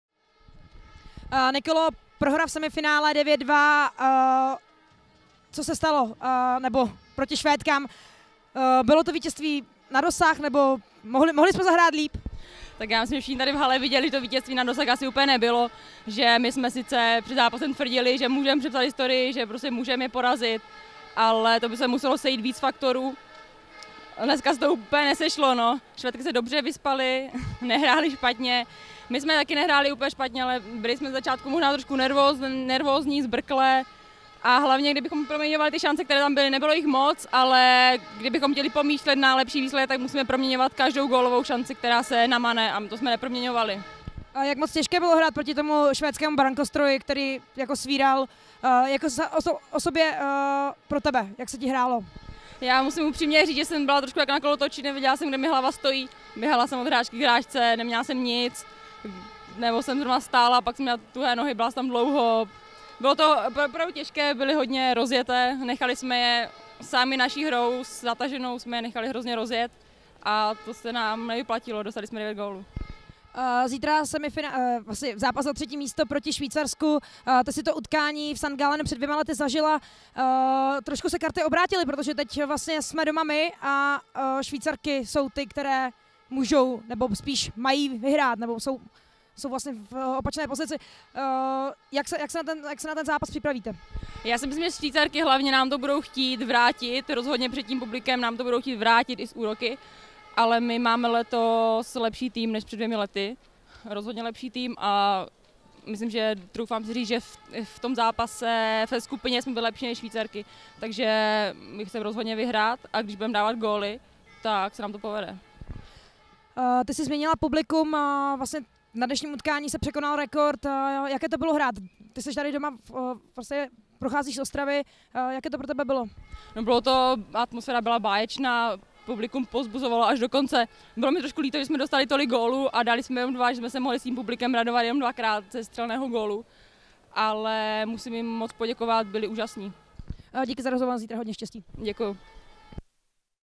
Interview SWE - CZE Nikola Černá
9th Women's World Floorball Championships
7th - 15th December 2013
Brno & Ostrava, Czech Republic